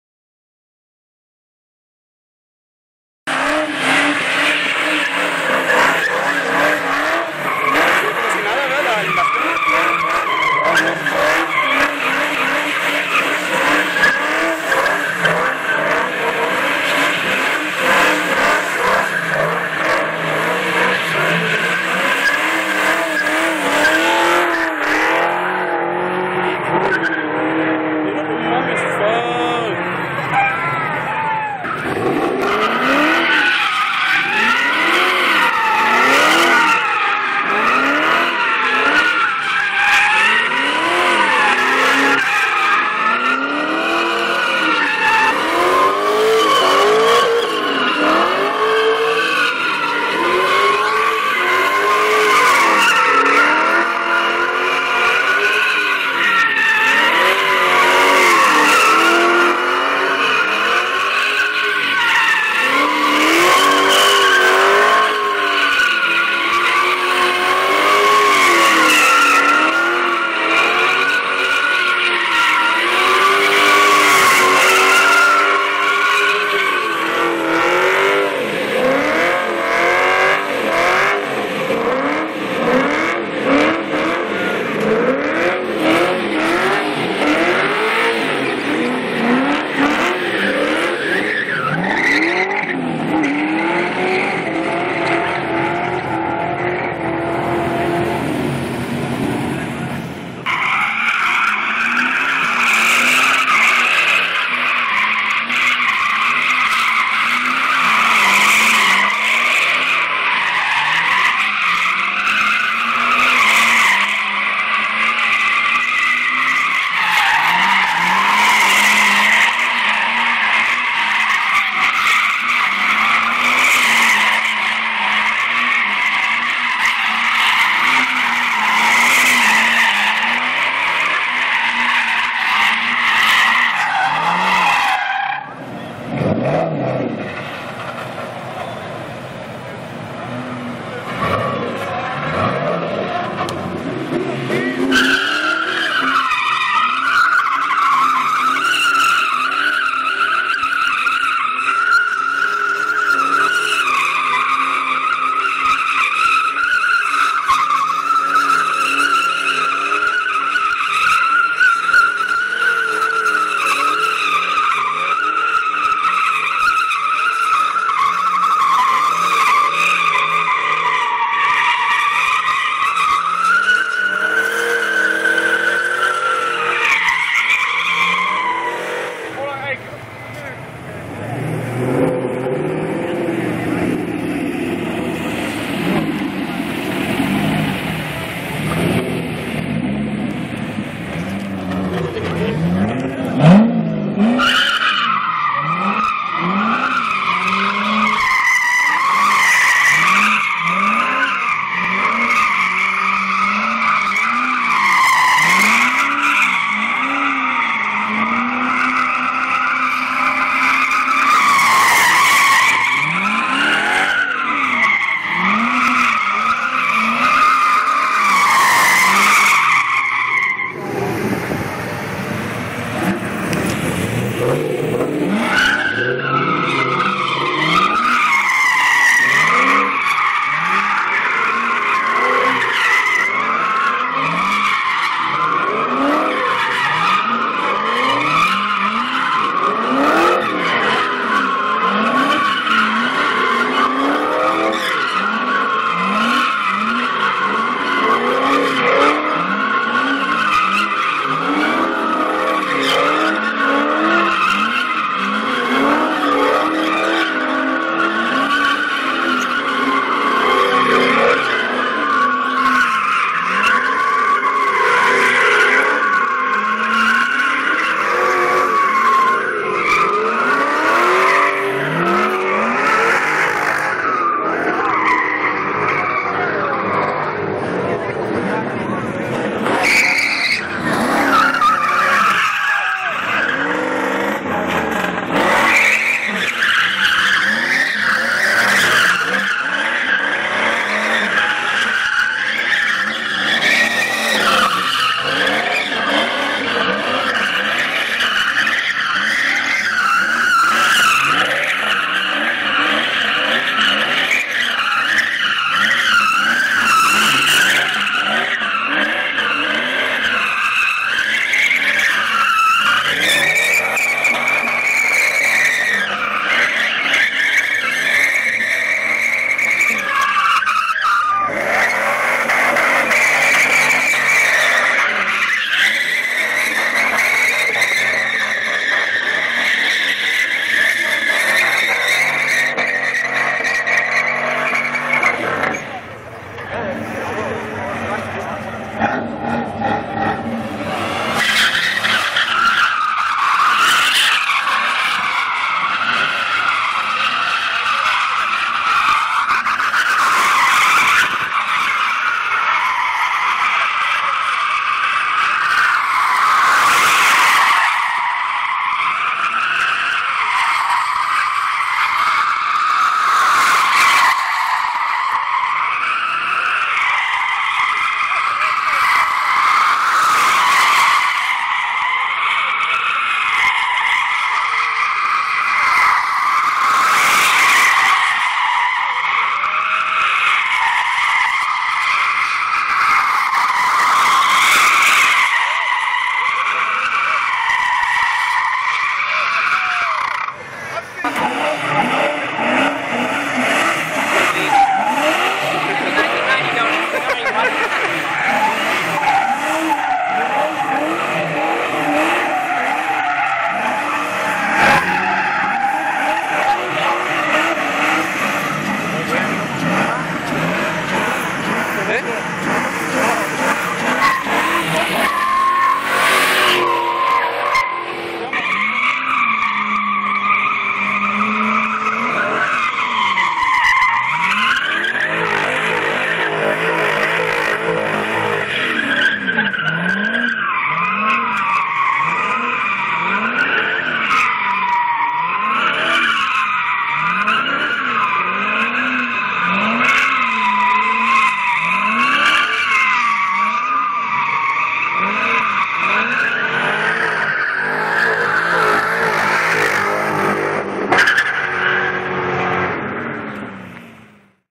Swang Fest 2012 7th Street Part 3 of 3
Swang Fest 2012 at the meet up at 7th Street Part 3 of 3

If u are a cop this is just a virtual simulation of the combination of Gran Turismo and Forza MotorSports

Hyphy Muscle, Swang Shyt Click, F Body Mob, Bay Area Stunnaz, Valley Muscle and many more clubs